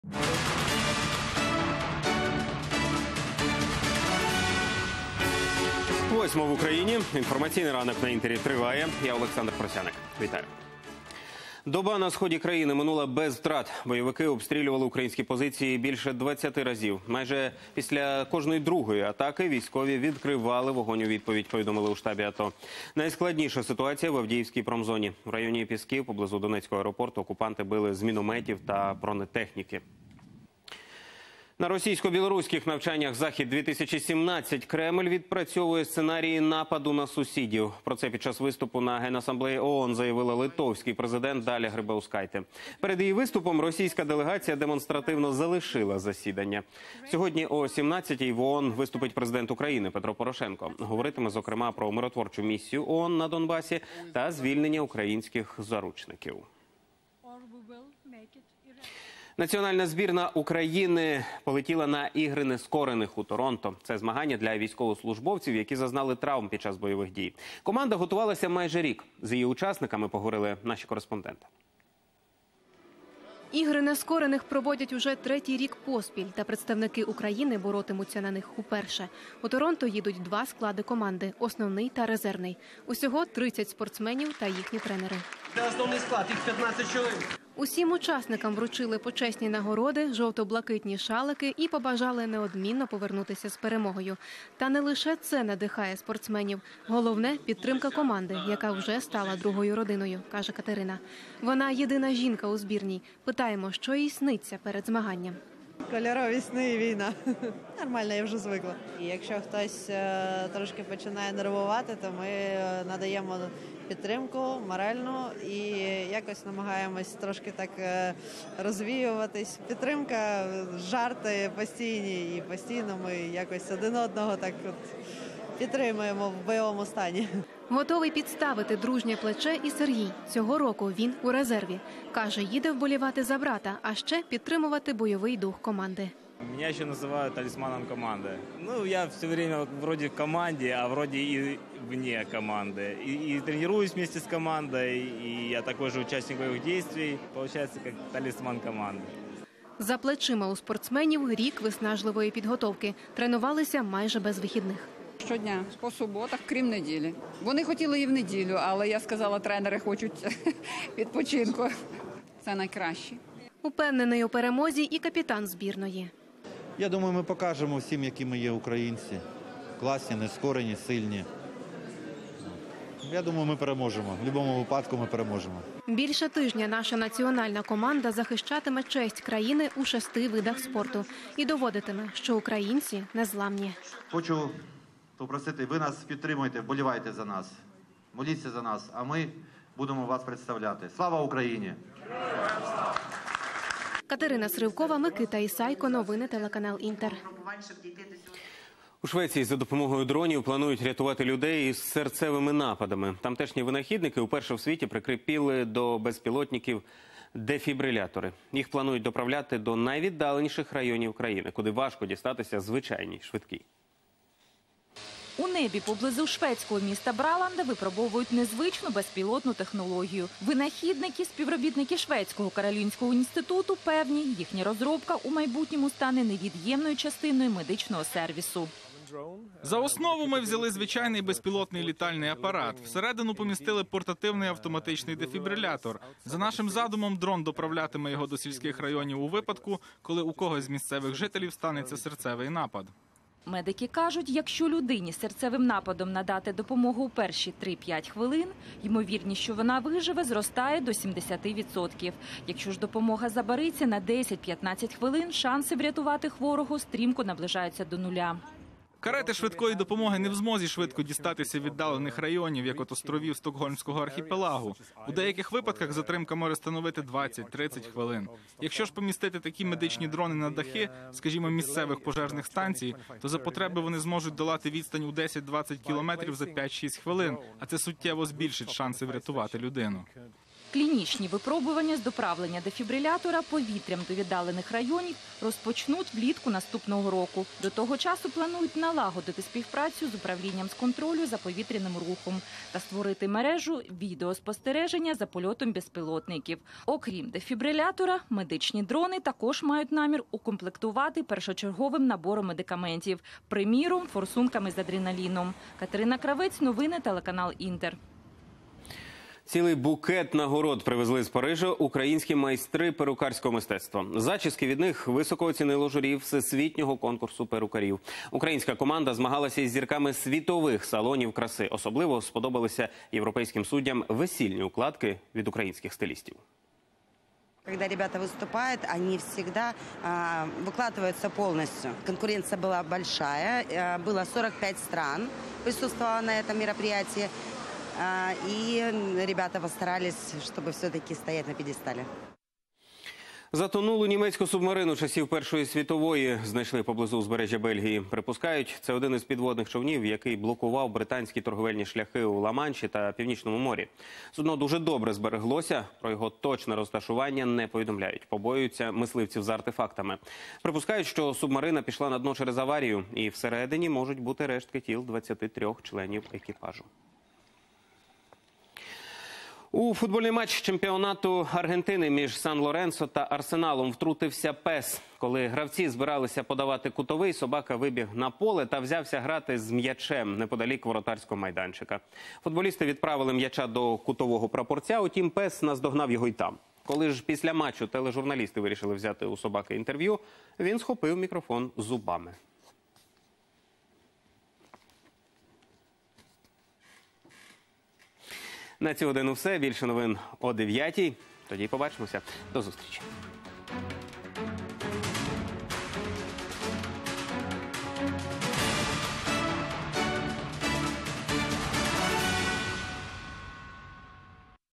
Новости 8:00, выпуск за 20 сентября 2017 года
Головні події на цю годину: Бойовики обстріляли Донецький аеропорт з важкої артилерії; Навчання "Захід-2017": Кремль відпрацьовує сценарії нападу на сусідів - Грибаускайте; "Ігри Нескорених" у Торонто: українські спортсмени відбули на змагання; У Швеції від серцевих нападів рятуватимуть дрони; Українські перукарі привезли з Парижу високі нагороди; Поблизу Бельгії знайшли німецьку субмарину часів Першої світової війни (відео); У Аргентині пес став зіркою футбольного матчу (відео).